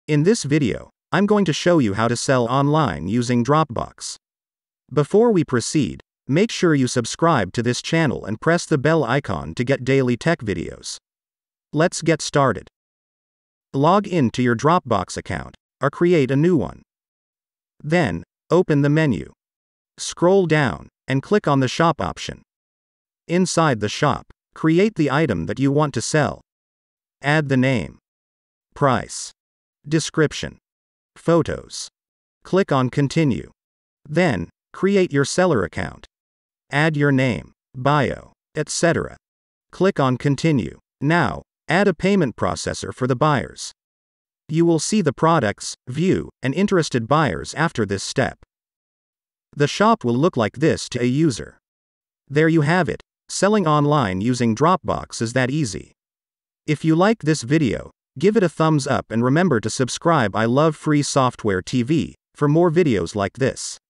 How to Sell Anything Online Using Dropbox
Log in to your Dropbox account, or create a new one.
Then, Open the menu.
Scroll down, and click on the shop option.
Inside the shop.
Create the item that you want to sell.
Add the name. Price. Description. Photos.
Click on Continue.
Then, create your seller account.
Add your name. Bio. Etcetera.
Click on continue.
Now, add a payment processor for the buyers.
You will see the products, view, and interested buyers after this step.
The shop looks will look like this to a user.

There you have it, selling online using Dropbox is that easy.